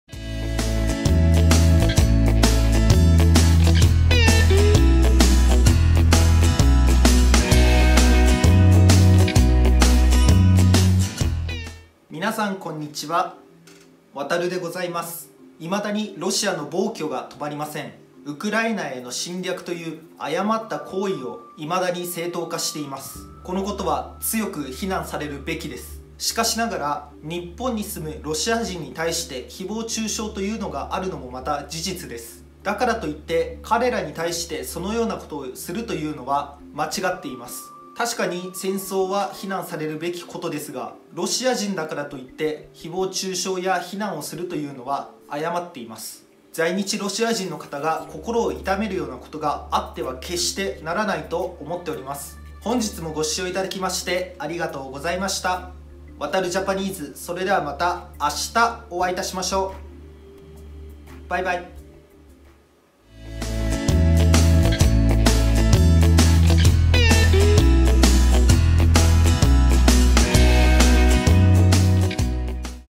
誹謗中傷は止めよう！
みなさん、こんにちは。ワタルでございます。今回は、ロシア軍の侵略に絡めて、日本に在住するロシア人の誹謗中傷を止めるよう訴えます。